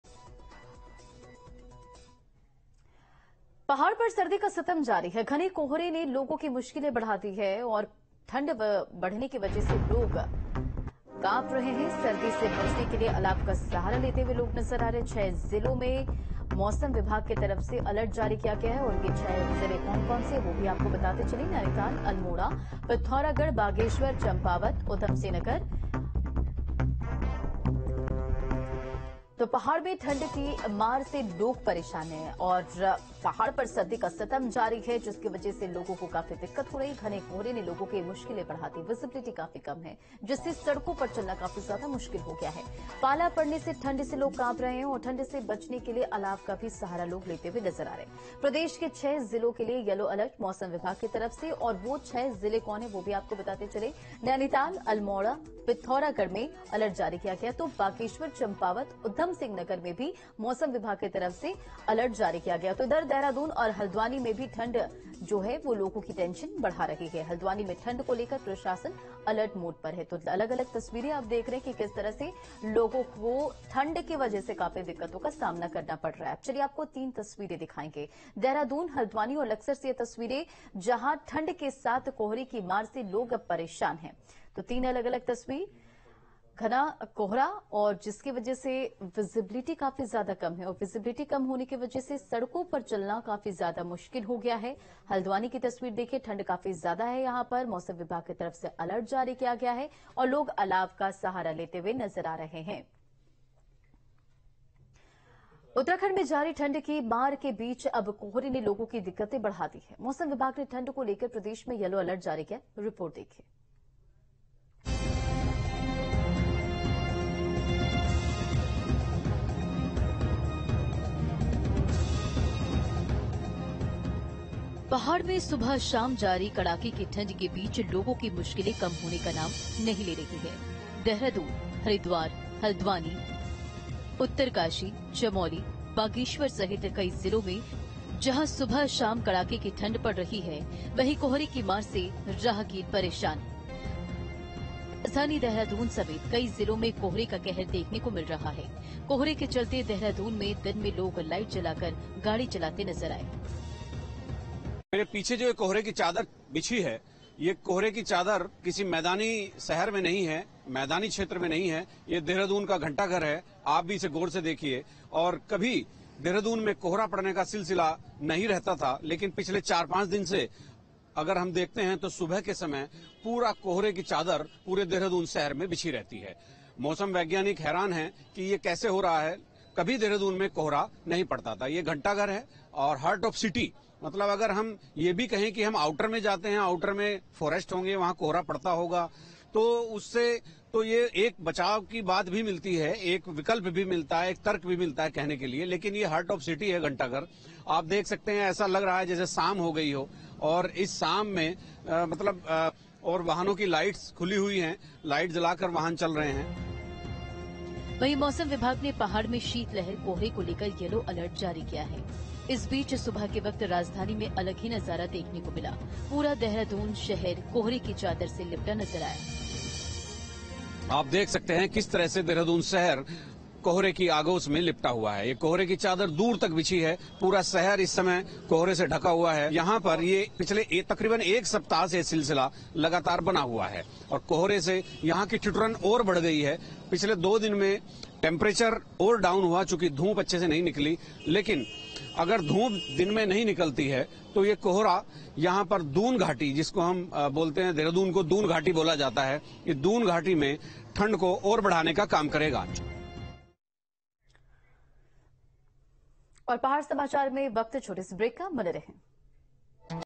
Weather Update : घने कोहरे ने बढ़ाई मुश्किलें, दो दिन में सर्दी और बढ़ने का अनुमान | Cold Wave | Fog
News State Apps डाउनलोड करें:
Download Android and iOS Apps for more updates: